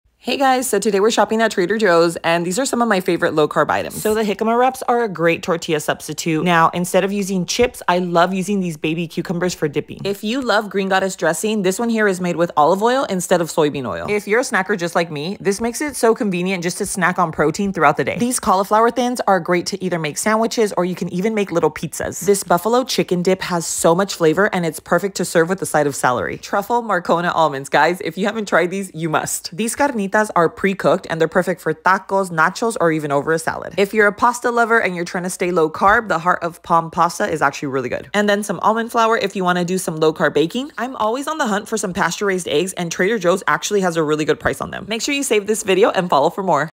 KETO/ LOW CARB TRADER JOES HAUL 🛒
Come with me to find some of my favorites at Trader Joes! 😋

🍳 My Favorite Air Fryer